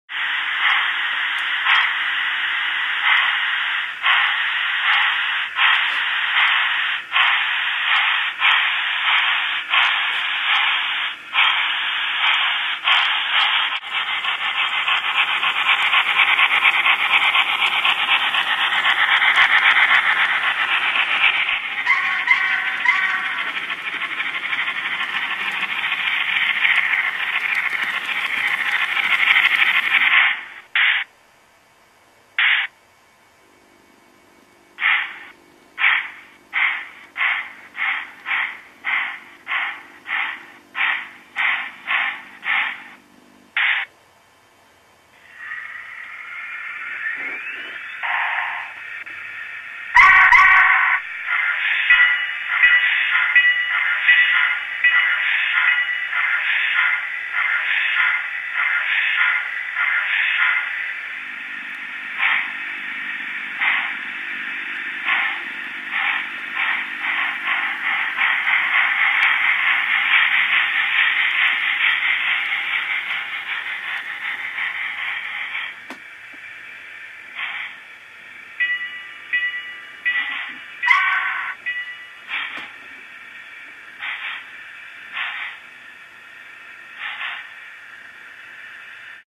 N-Scale Walthers Y3 2-8-8-2
Test run of my new N-Scale Y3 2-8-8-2 Steam Locomotive, with my new PowerCab Controller. I'm new to Model Railroading and decided to go DCC. Pretty exciting stuff. Layout is a modified version of Woodland Scenics Scenic Ridge with Kato Unitrack